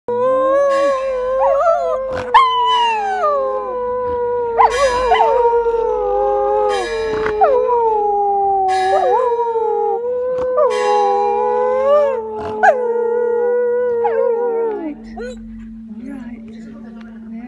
Gordo and Jefe the Samoyeds, howling with the Church bells
Gordo and Jefe, calling the faithful!